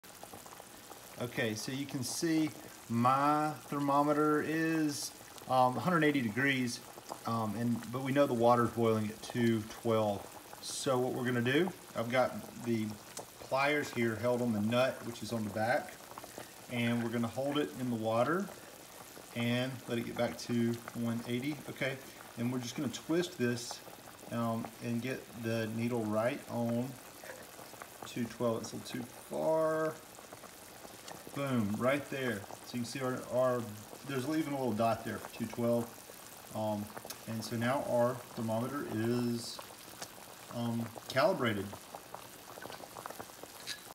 How to Calibrate The Thermometer on a Big Green Egg or Kamado Joe
Quick video showing how to calibrate the thermometer on a Big Green Egg, Kamado Joe, or any other grill.